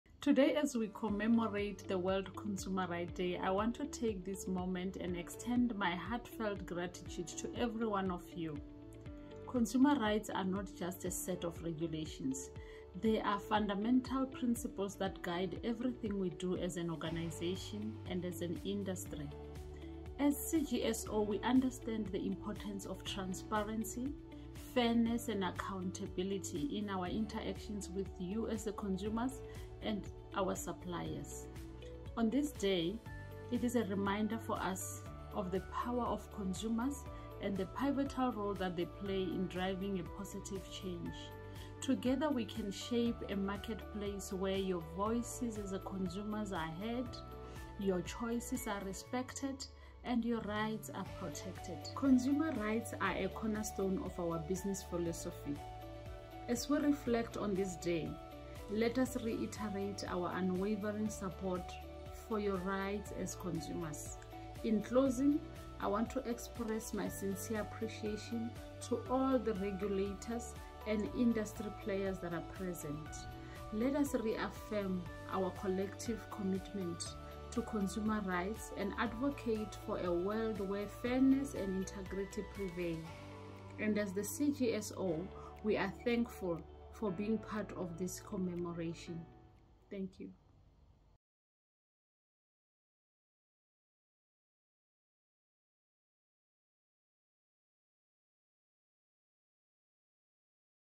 CGSO CEO Queen Munyai highlights the importance of safe products on World Consumer Rights Day 2024
CEO Ms Queen Munyai giving a message of support to all regulators and consumer bodies that advocate on safe  products to keep consumers safe, as we Celebrate World Consumer Rights Day 2024